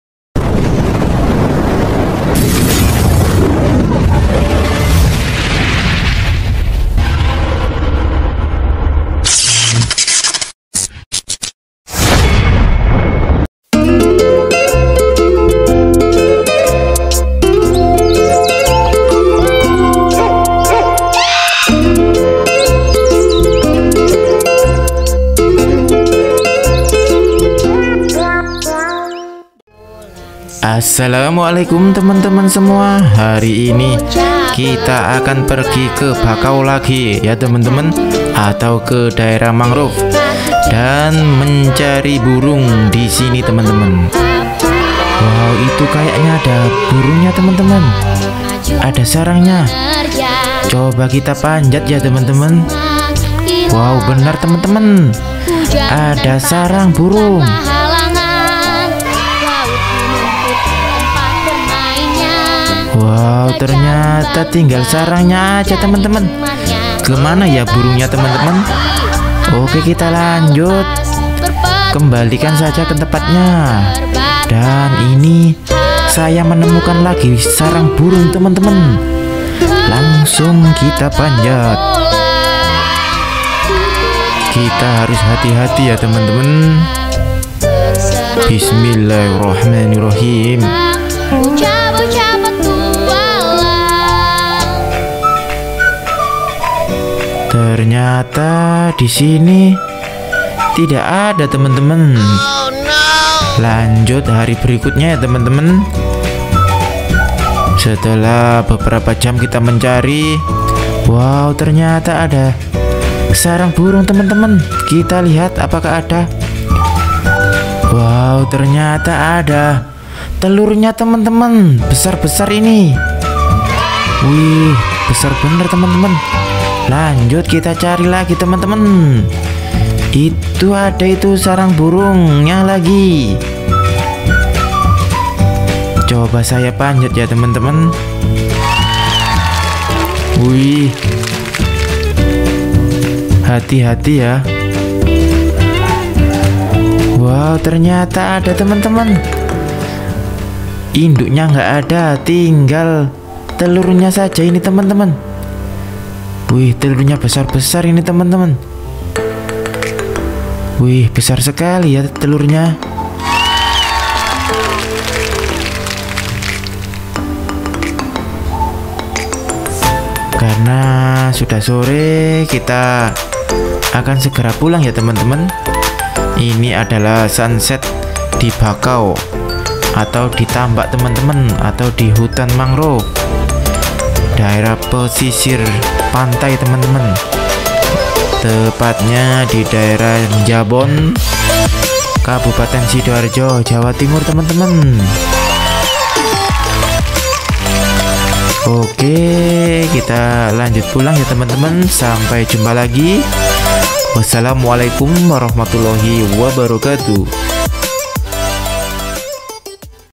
Berhasil Menemukan Telur Burung Raksasa Di Hutan Mangrove Dekat Pesisir Pantai
VIDEO INI SYUTING DIKAWASAN DAERAH PESISIR MANGROVE-JABON,SIDOARJO_JAWA TIMUR